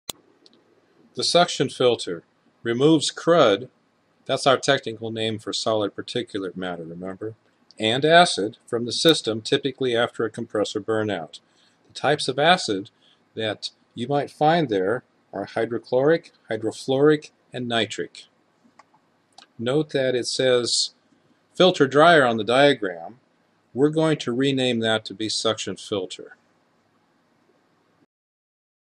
Suction Filter
Introduction to Air Conditioning & Refrigeration at Riverside Community College - Lecture by Neil Orme